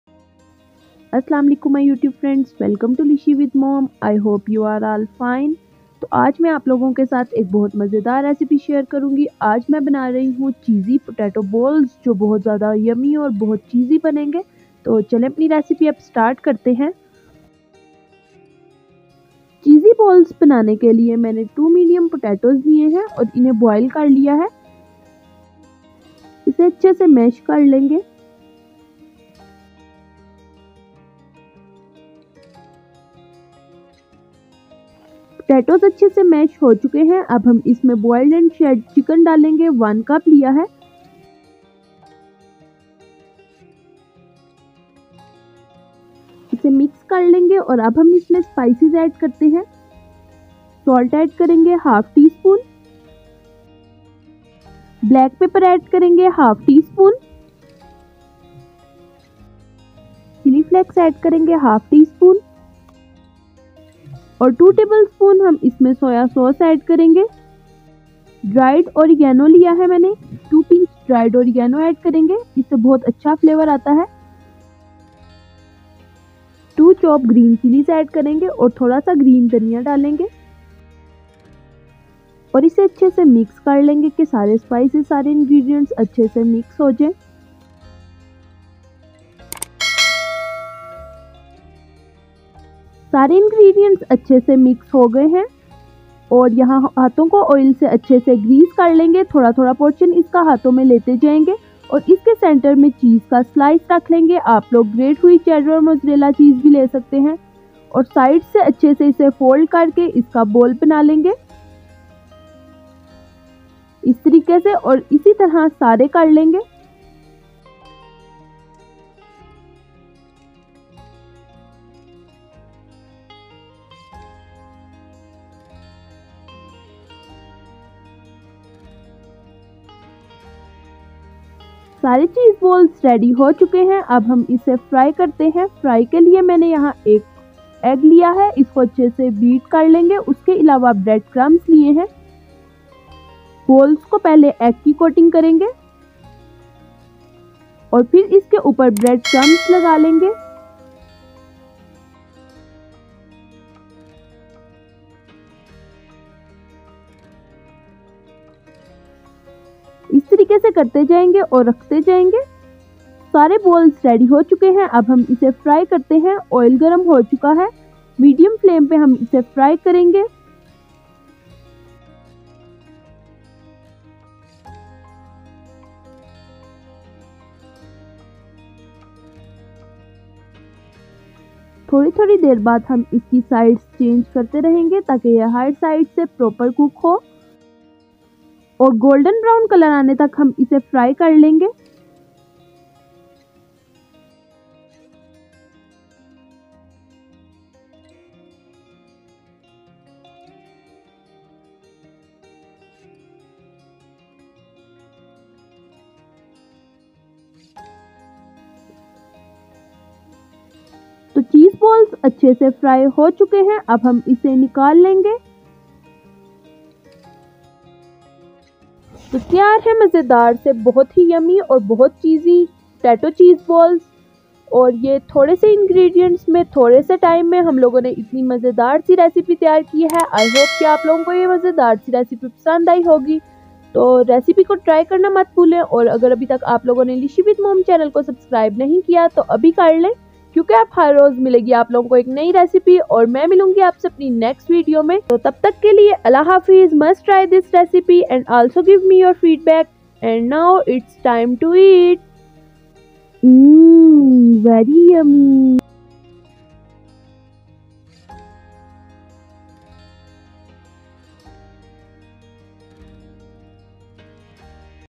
Cheese Balls | Cheesy Snacks | 5 Minutes Snacks Recipe by Lishee with Mom
Today in this video you will get the recipe of Cheese Balls | Cheesy Snacks | 5 Minutes Snacks Recipe by Lishee with Mom
You just need few ingredients to make this yumiests snacks.